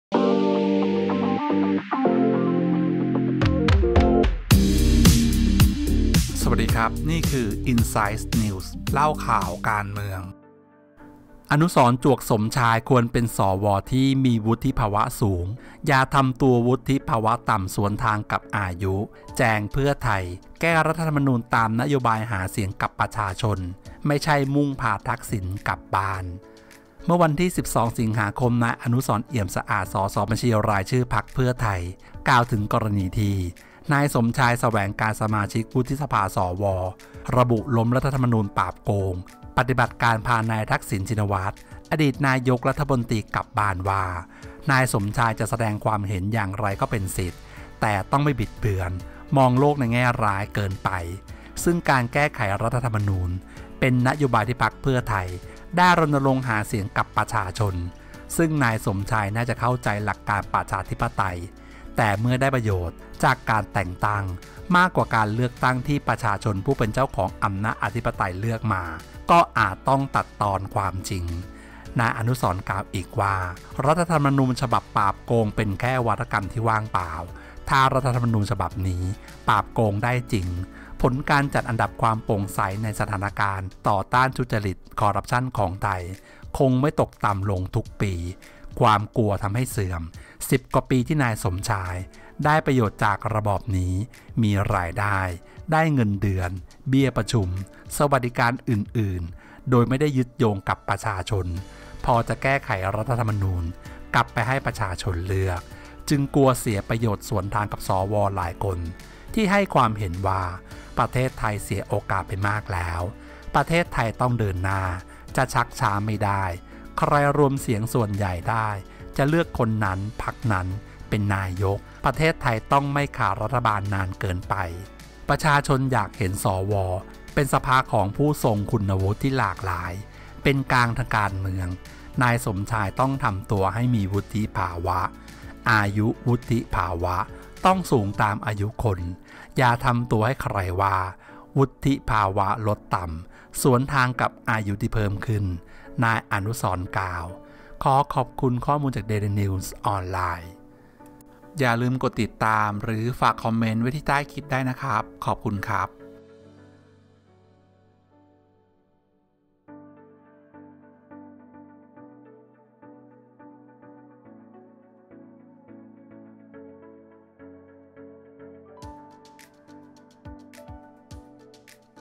จี้ใจดำ!!! "อนุสรณ์" จวก "สมชาย" เป็น ส.ว. อย่าทำตัววุฒิภาวะต่ำ สวนทางกับอายุที่เพิ่มขึ้น |อินไซด์
‘อนุสรณ์’จวก‘สมชาย’เป็น ส.ว.อย่าทำตัววุฒิภาวะต่ำ|อินไซด์นิวส์ไทย
‘อนุสรณ์’ จวก ‘สมชาย’ ควรเป็น สว. ที่มีวุฒิภาวะสูง อย่าทำตัววุฒิภาวะลดต่ำสวนทางกับอายุ แจง ‘เพื่อไทย’ แก้รัฐธรรมนูญตามนโยบายหาเสียงกับประชาชน ไม่ใช่มุ่งพา ‘ทักษิณ’ กลับบ้าน
เมื่อวันที่ 12 ส.ค. นายอนุสรณ์ เอี่ยมสะอาด สส.บัญชีรายชื่อ พรรคเพื่อไทย (พท.) กล่าวถึงกรณีที่นายสมชาย แสวงการ สมาชิกวุฒิสภา (สว.) ระบุล้มรัฐธรรมนูญปราบโกง ปฏิบัติการพานายทักษิณ ชินวัตร อดีตนายกรัฐมนตรี กลับบ้าน ว่า นายสมชายจะแสดงความเห็นอย่างไรก็เป็นสิทธิ แต่ต้องไม่บิดเบือน หรือมองโลกในแง่ร้ายเกินไป ซึ่งการแก้ไขรัฐธรรมนูญ เป็นนโยบายที่พรรคเพื่อไทยได้รณรงค์หาเสียงกับประชาชน ซึ่งนายสมชายน่าจะเข้าใจหลักการประชาธิปไตย แต่เมื่อได้ประโยชน์จากการแต่งตั้ง มากกว่าการเลือกตั้งที่ประชาชนผู้เป็นเจ้าของอำนาจอธิปไตยเลือกมา ก็อาจต้องตัดตอนความจริง
นายอนุสรณ์ กล่าวอีกว่า รัฐธรรมนูญฉบับปราบโกงเป็นแค่วาทกรรมที่ว่างเปล่า ถ้ารัฐธรรมนูญฉบับนี้ปราบโกงได้จริง ผลการจัดอันดับความโปร่งใสสถานการณ์ต่อต้านทุจริตคอร์รัปชันของไทย คงไม่ตกต่ำลงทุกปี ความกลัวทำให้เสื่อม 10 กว่าปีที่นายสมชายได้ประโยชน์จากระบอบนี้ มีรายได้ ได้เงินเดือน เบี้ยประชุม สวัสดิการอื่นๆ โดยไม่ได้ยึดโยงกับประชาชน พอจะแก้ไขรัฐธรรมนูญ กลับไปให้ประชาชนเลือก จึงกลัวเสียประโยชน์ สวนทางกับ สว. หลายคน ที่ให้ความเห็นว่าประเทศไทยเสียโอกาสไปมากแล้ว ประเทศไทยต้องเดินหน้า จะชักช้าไม่ได้ ใครรวมเสียงส่วนใหญ่ได้ จะเลือกคนนั้นพรรคนั้นเป็นนายกฯ ประเทศไทยต้องไม่ขาดรัฐบาลนานเกินไป
“ประชาชนอยากเห็น สว. เป็นสภาของผู้ทรงคุณวุฒิที่หลากหลาย เป็นกลางทางการเมือง นายสมชายต้องทำตัวให้มีวุฒิภาวะ อายุวุฒิภาวะต้องสูงตามอายุคน อย่าทำตัวให้ใครว่า วุฒิภาวะลดต่ำ สวนทางกับอายุที่เพิ่มขึ้น”นายอนุสรณ์ กล่าว

เดลินิวส์ dailynews